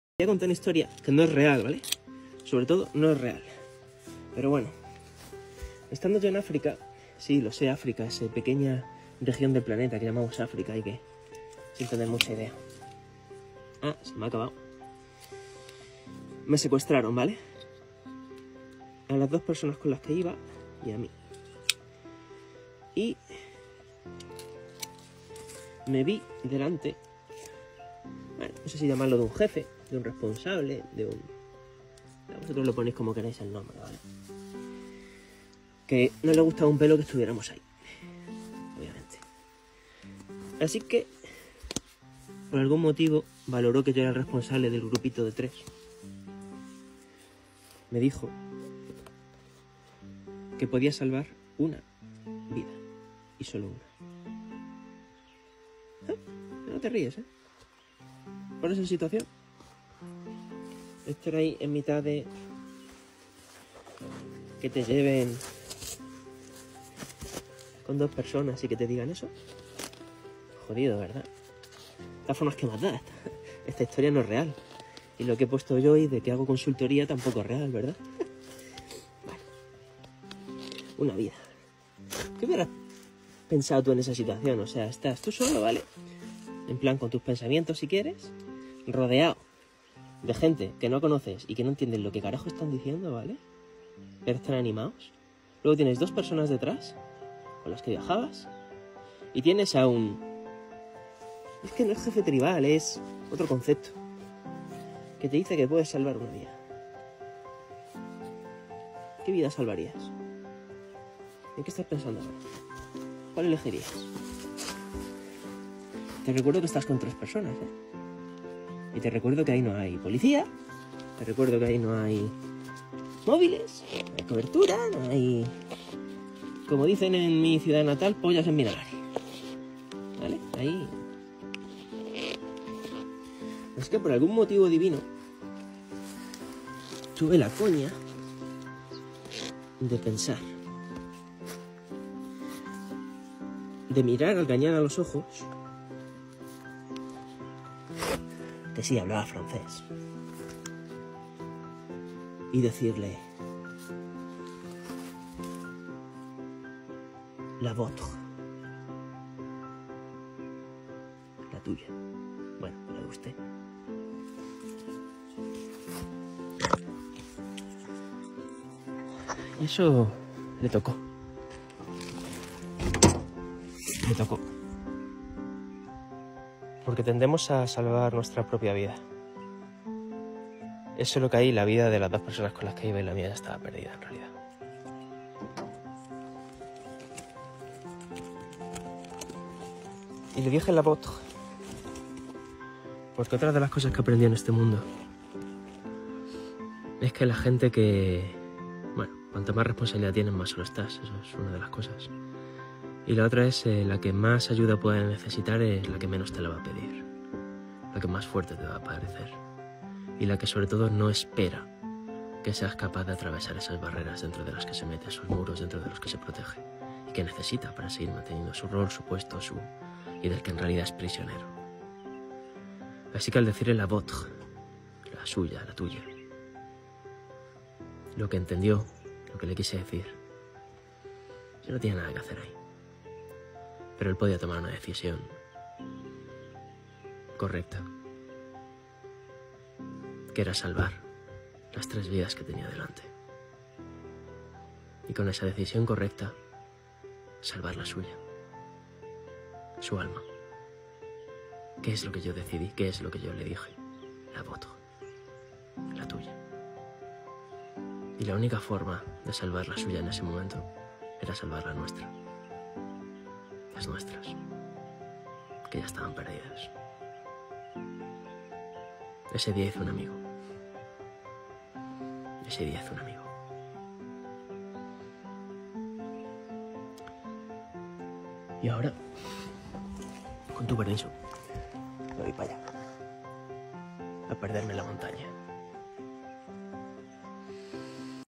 Secuestro en África - Debería Compartir Historias Reales Protegiendo Protagonistas ?
Te gusta la historia !? Quieres que cuente más !? La historia es inventada no te la creas.. tan inventada como jajajaja venga ahora en serio.. la historia no es real, vale !? No te la creas !! Ahora bien.. quieres que cuente más historias !? Reales digo !? Como la del vídeo que digo que no es real digo !? El problema de las historias es proteger a sus protagonistas.. por eso es más fácil contar ficción que realidad.. a que se me da bien !? Proteger a los protagonistas digo !! Jajajajaja.. tanto escribir no me ha quedado claro si te gustan las historias o no.. pero ya sabes que siempre (*casi que a veces patino!!) te leo.. ahh y se muchas historias.. y no porque las haya leído pues tengo mala memoria.. me encanta TikTok